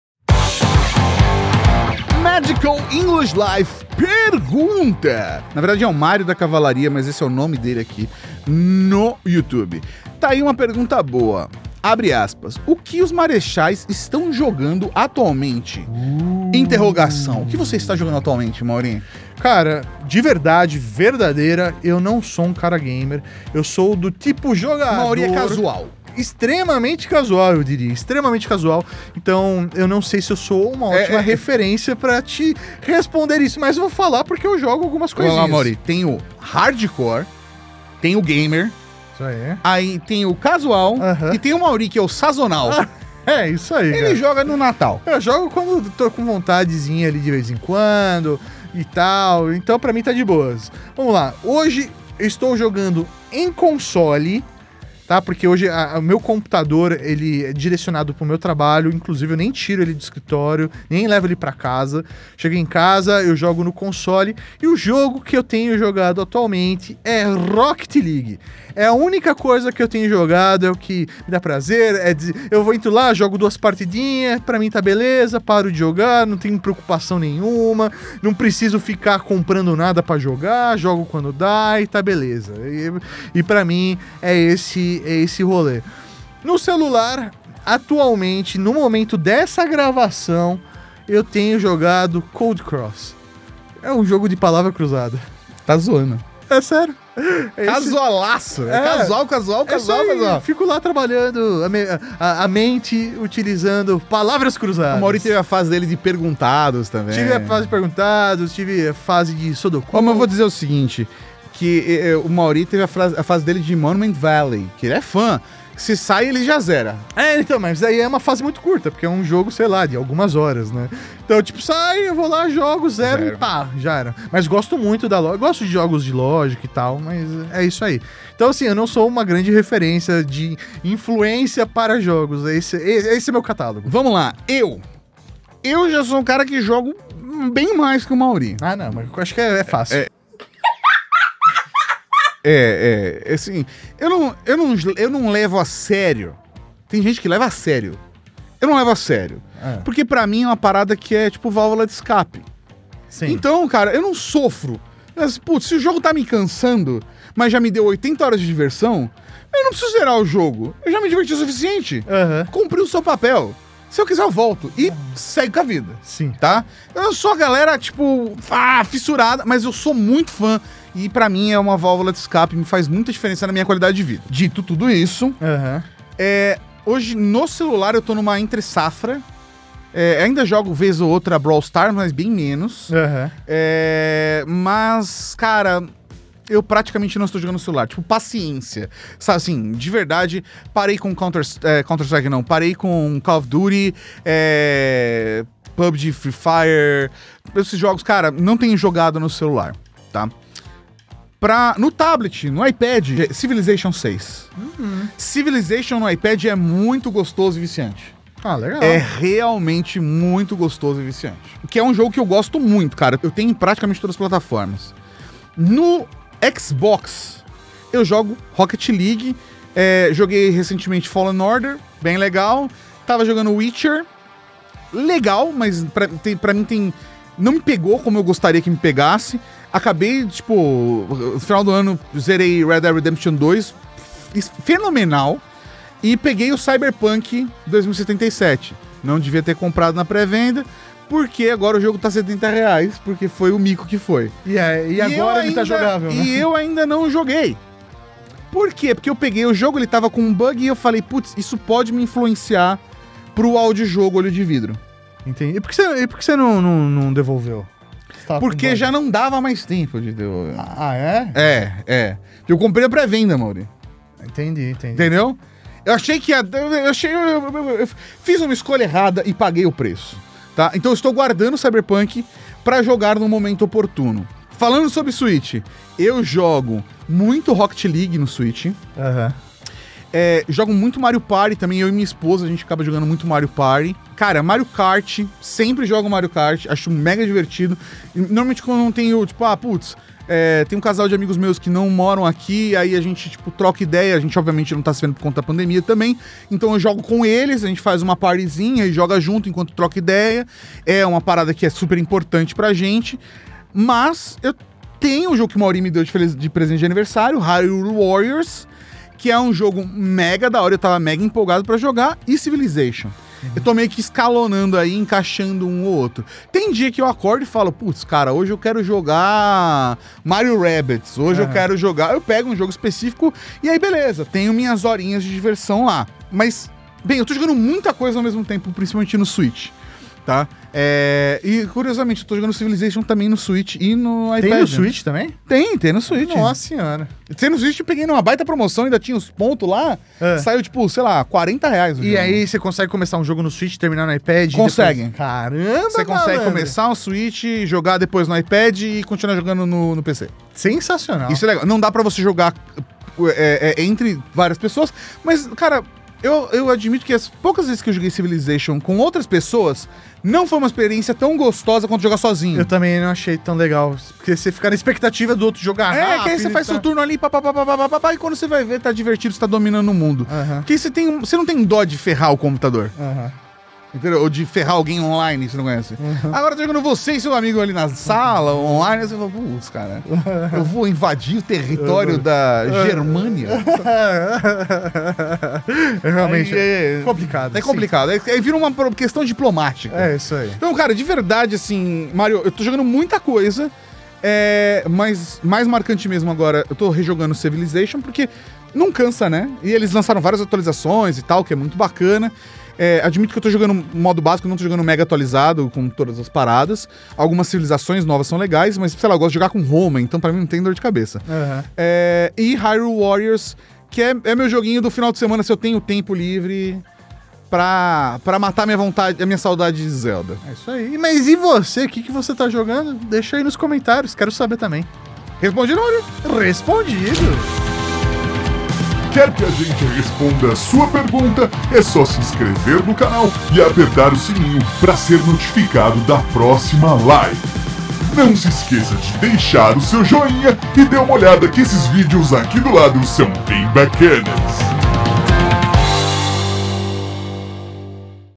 O que nós estamos JOGANDO? RESPONDIDO!
O que os Marechais da Cavalaria Geek estão jogando atualmente? O Magical English Life fez essa pergunta pra gente... Veja nossa resposta definitiva para o dilema!